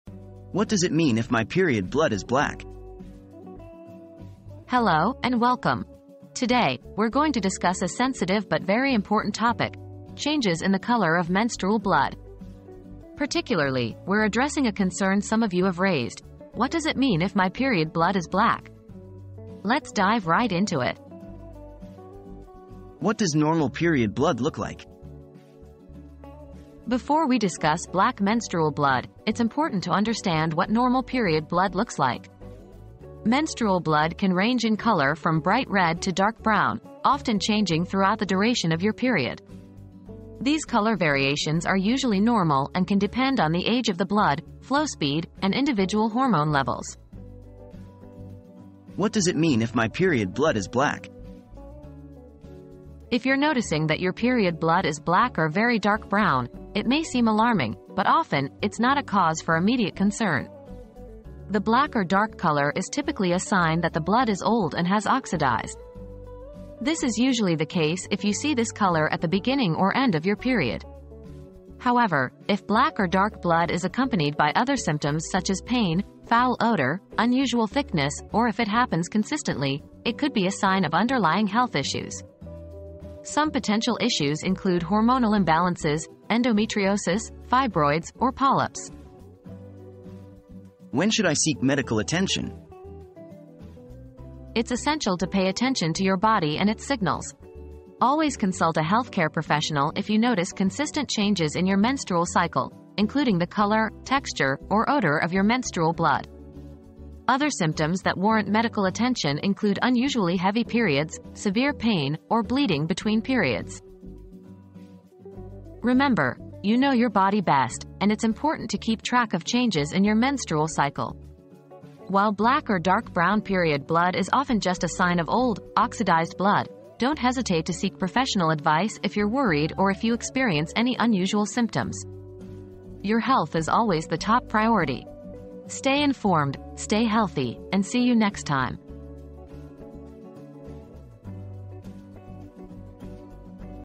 What does it mean if my period blood is black?
Understanding Your Menstrual Cycle: Why Is My Period Blood Black? • Is black period blood normal? Find out in this informative video! Learn about the different colors of menstrual blood and what they mean. Discover why your period blood may turn black and when it's time to seek medical attention. Stay informed and take charge of your menstrual health!

00:00 • What does it mean if my period blood is black?
00:22 • What does Normal Period Blood Look Like?
00:48 • What does it Mean if my Period Blood is Black?
01:30 • When Should I Seek Medical Attention?